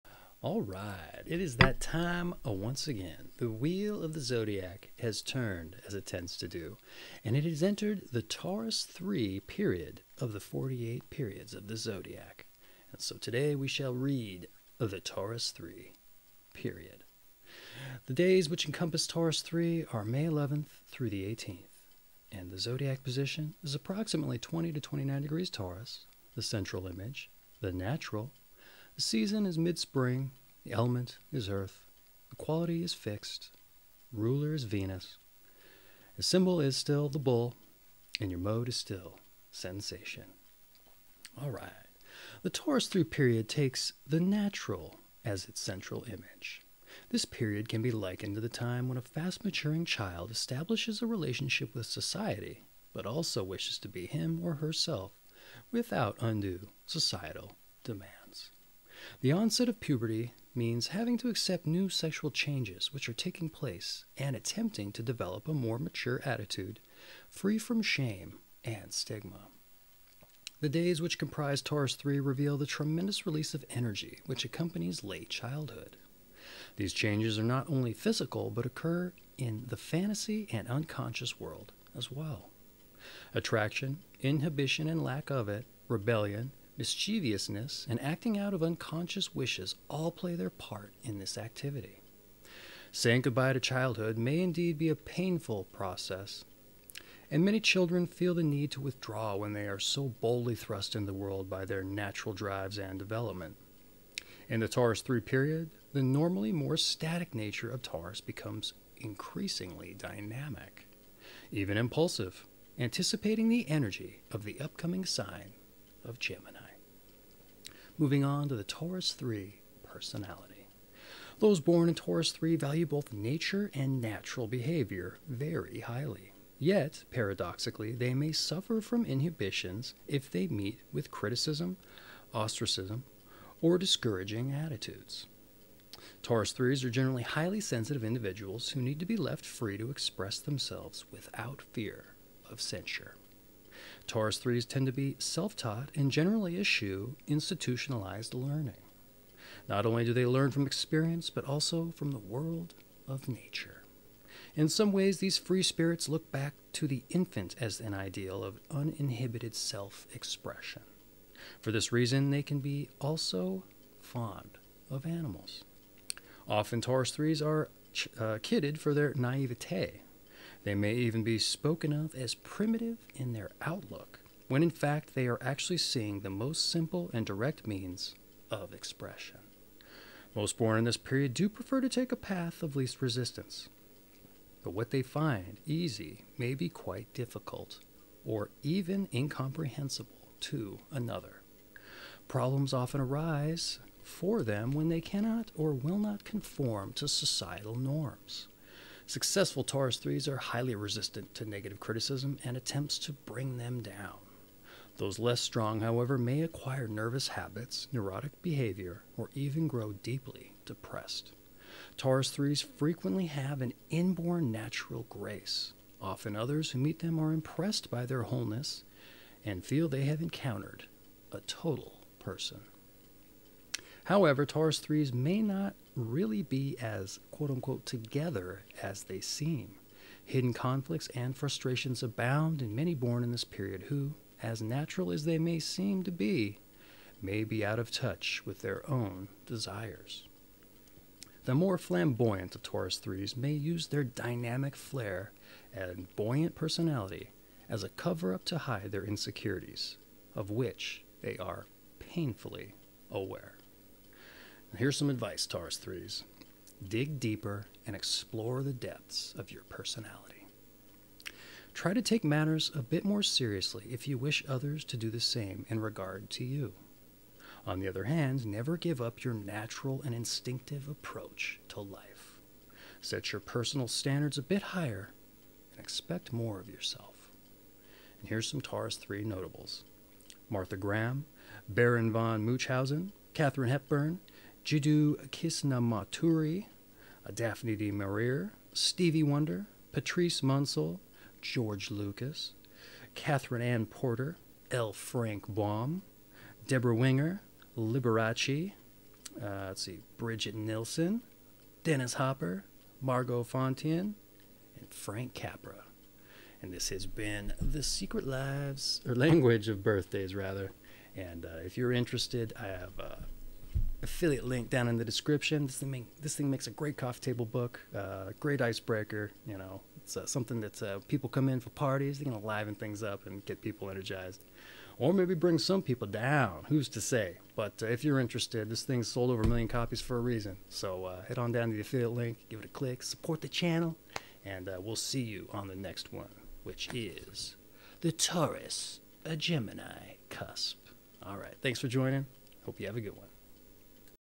'Secret Language of Birthdays' Taurus III (May 11 - 18) Breakdown ♉☝️✌️🐂
Today we explore:
The Taurus III Period  ♉☝️✌️🐂
Element/Quality: Fixed Earth 🔧 🪨
Central Image: The Natural 💁‍♂️
Season: Mid Spring 🌱
Ruler: Venus
Symbol: The Bull 🐂
Mode: Sensation 💆

Want you your own copy of The Secret Language of Birthday's
by Gary Goldschneider & Joost Elffers?

For the complete stream and more head over to the DykASSt Zen Cast @

00:00 - Intro
00:42 - Taurus III
01:57 - Taurus III Personality
04:14 - Advice
04:38 - Taurus III Notables
05:06 - Outro